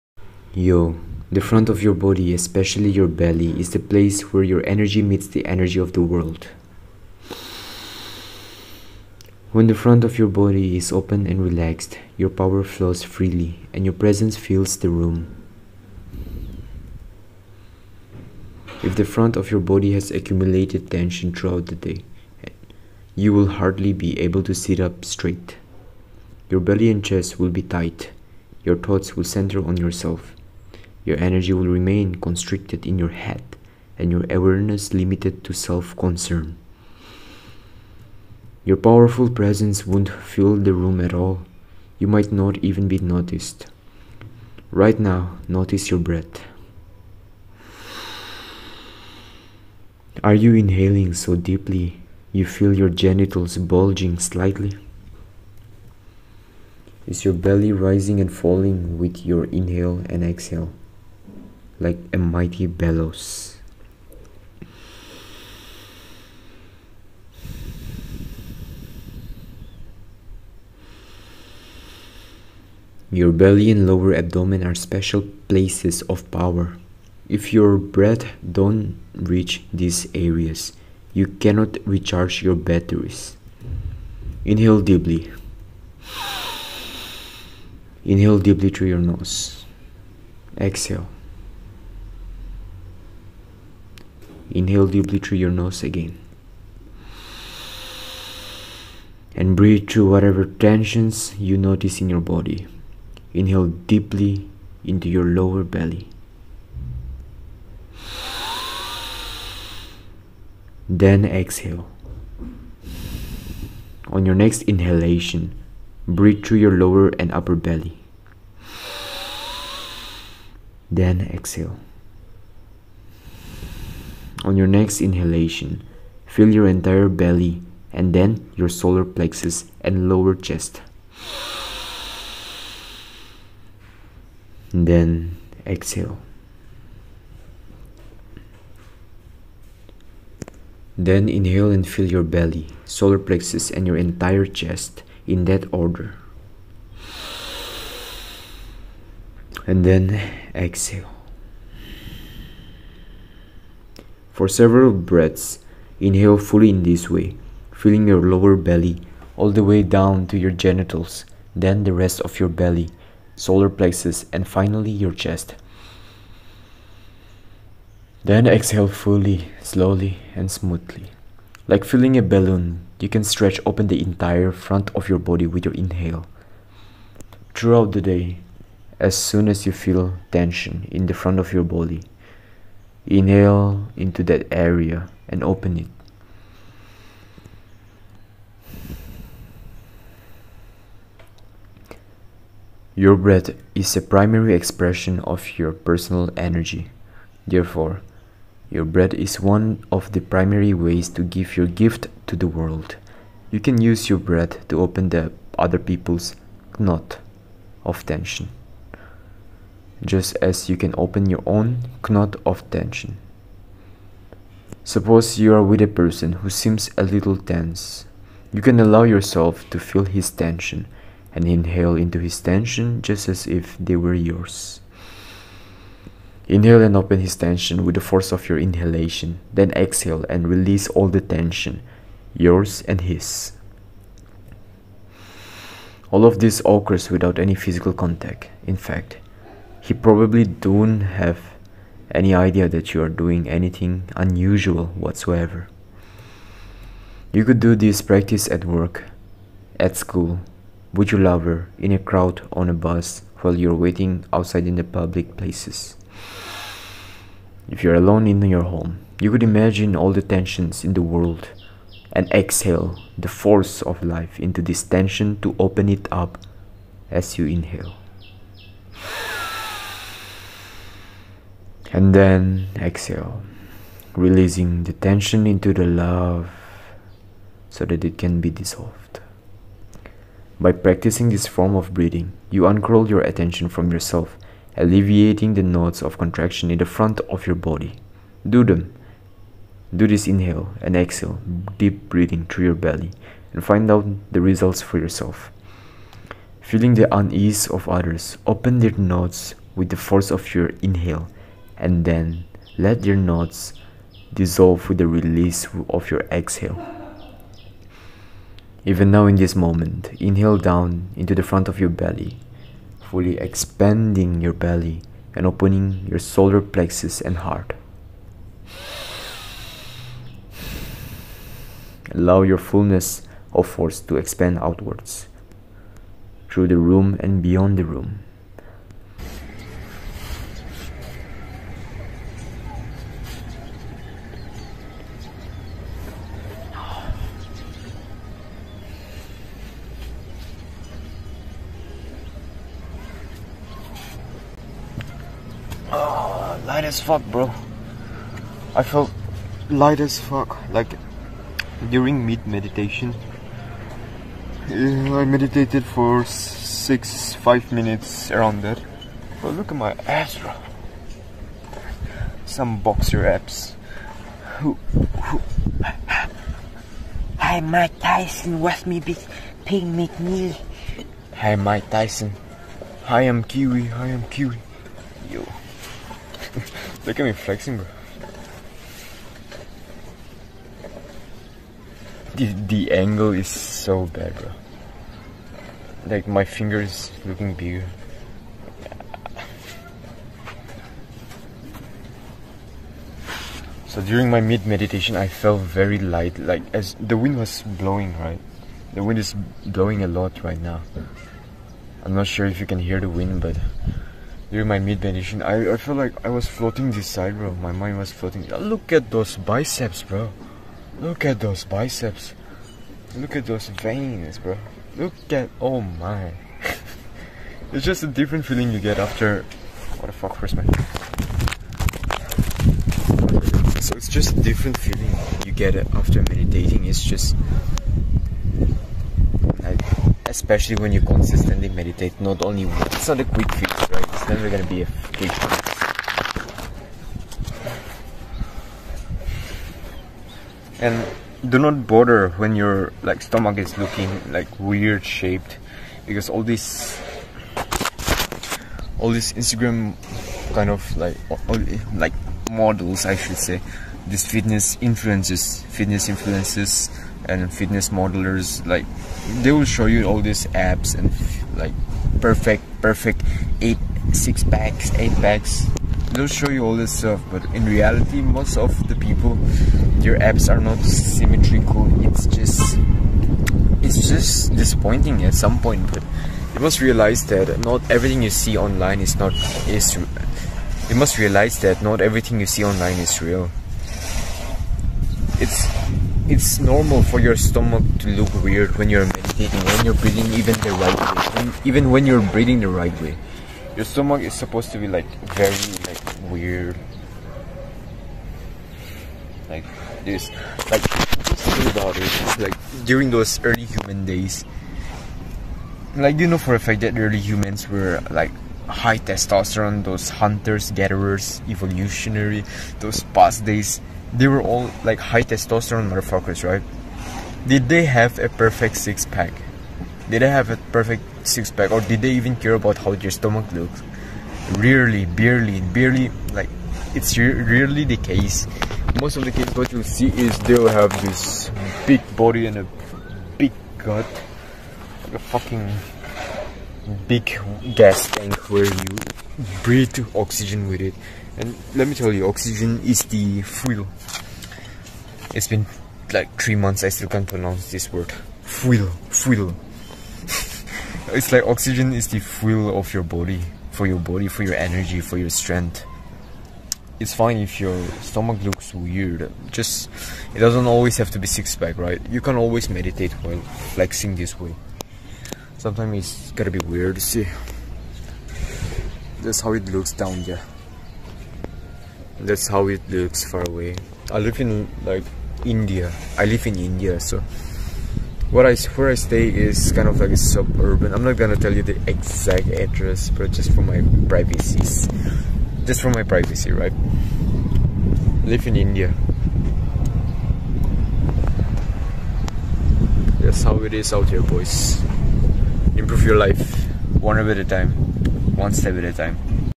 How to Breathe Like A Superior Man
Unstimulate yourself.

Voiceover of the Chapter "breathe down the front" from the book: The Way Of The Superior Man.

Improve your life